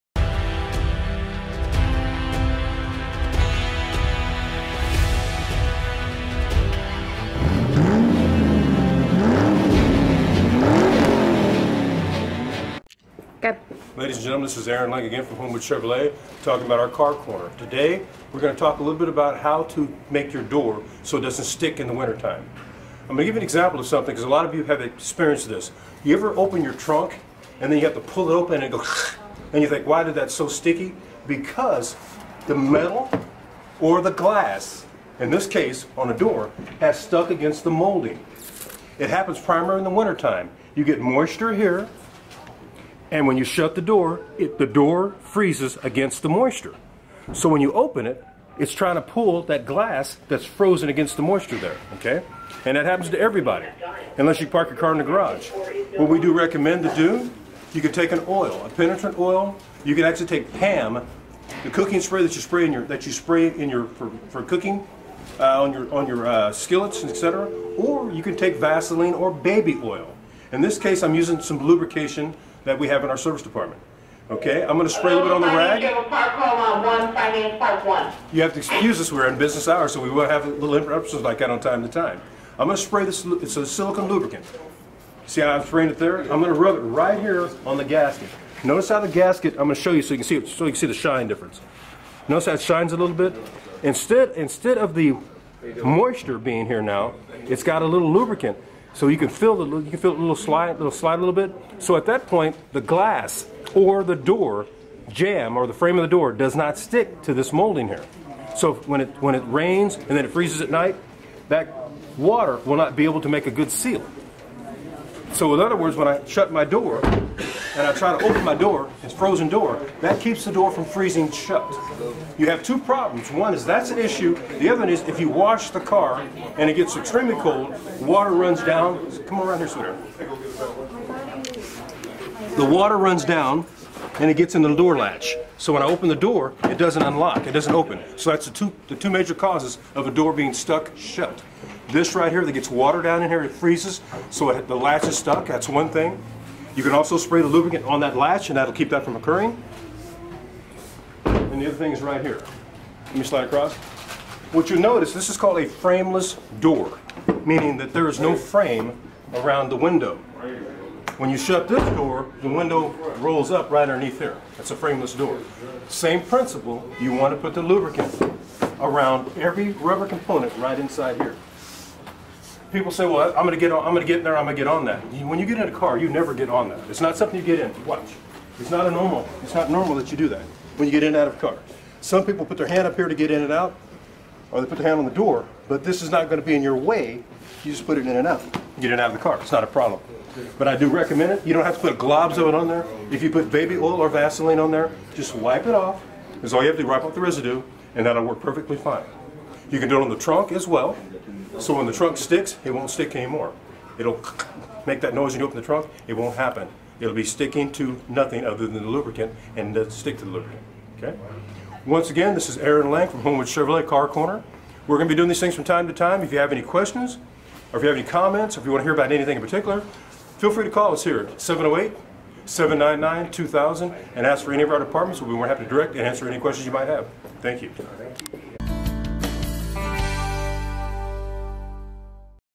HOW TO PREVENT YOUR DOORS FROM FREEZING UP | Car Corner
Here's another episode of Car Corner with Aaron Lang. He is showing you How to prevent your doors from freezing up. A simple process you can follow for car safety.